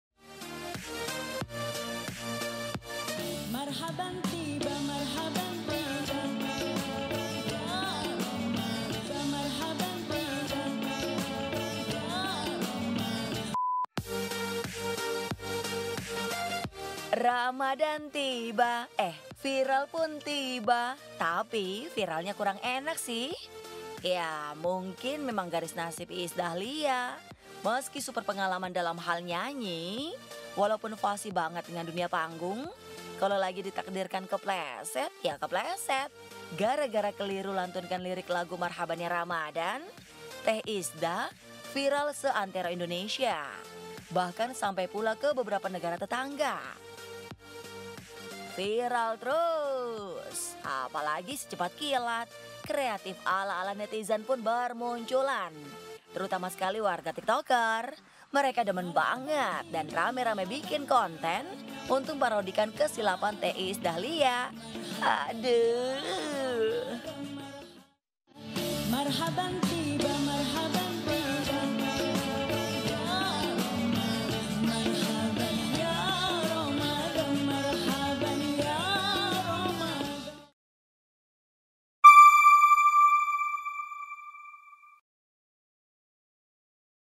Ini Imbas Iis Dahlia Kepeleset Lirik Lagu
Iis Dahlia juga manusia, itu hal utama yang perlu disadari warganet harusnya.